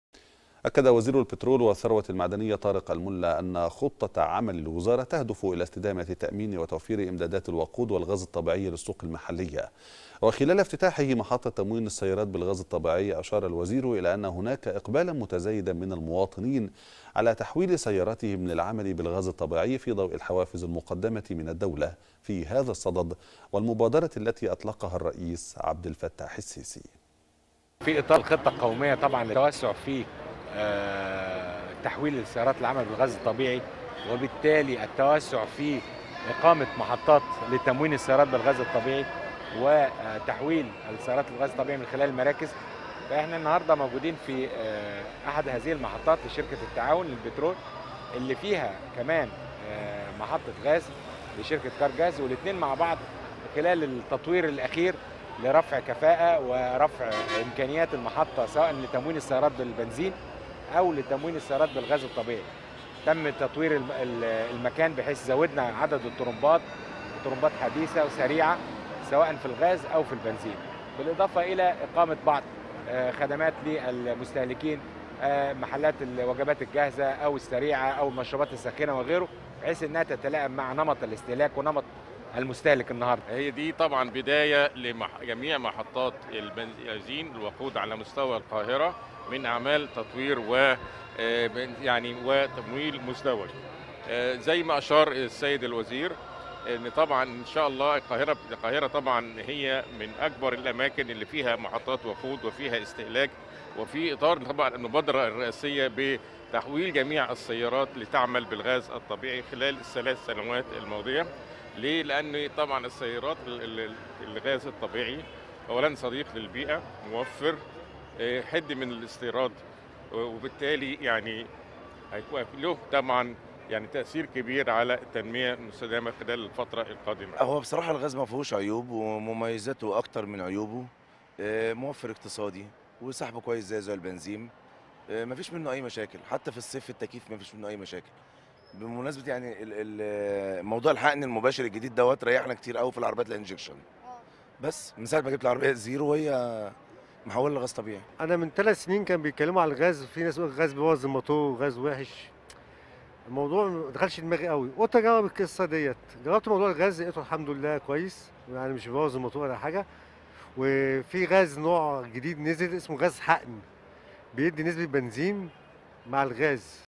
وزير البترول يفتتح أحدث محطة وقود متكاملة لتموين السيارات بالغاز الطبيعي والوقود السائل بعد تطويرها
أخبار_TeN - افتتح المهندس طارق الملا، وزير البترول والثروة المعدنية يرافقه اللواء خالد عبد العال محافظ القاهرة محطة تموين السيارات بالغاز الطبيعي التابعة لشركة «كارجاس» بعد إعادة بنائها وتجديدها داخل محطة الاستاد التابعة لشركة التعاون للبترول، وهو التطوير الذي نفذته شركة غاز مصر، وتقدم المحطة خدمات متكاملة للمواطنين تتضمن التموين بالوقود السائل والغاز الطبيعي والشحن الكهربائي.